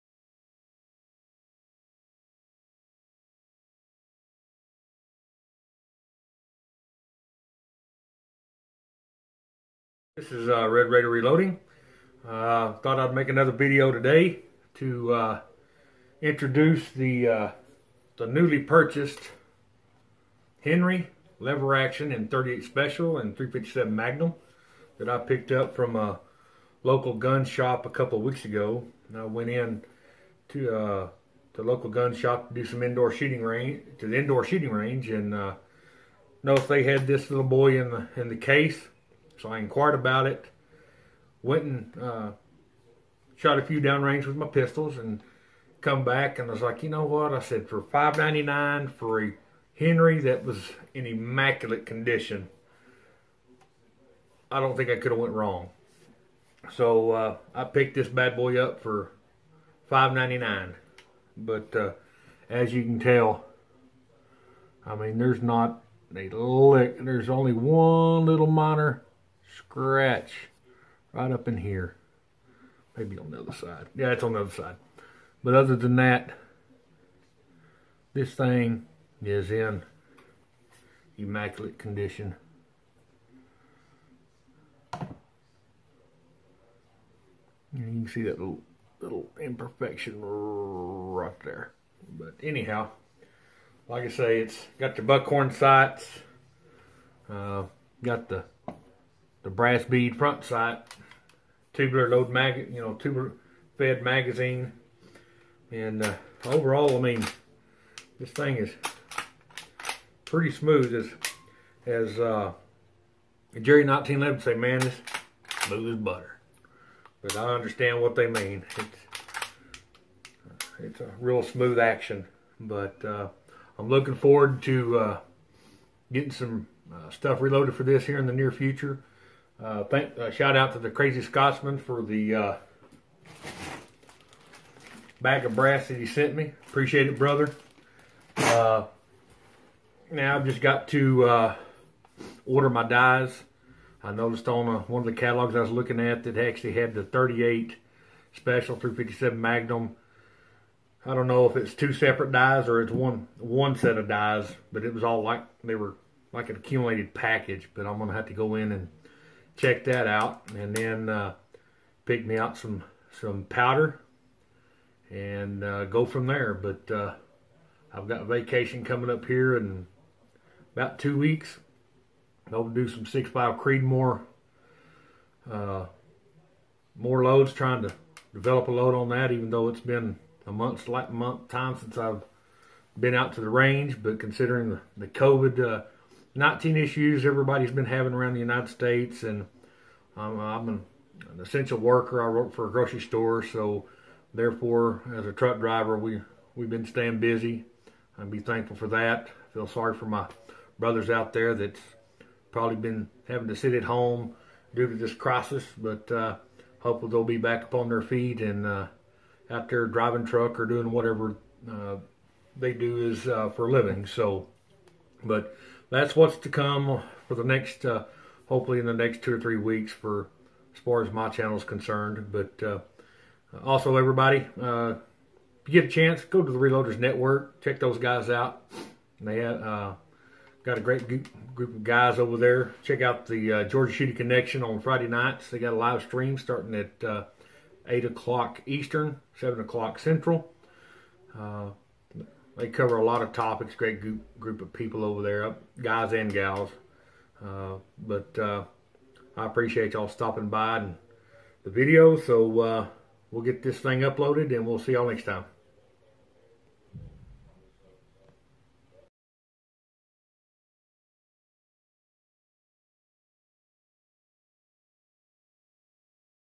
Henry lever action
Goin over my Henry lever action in 38/357 magnum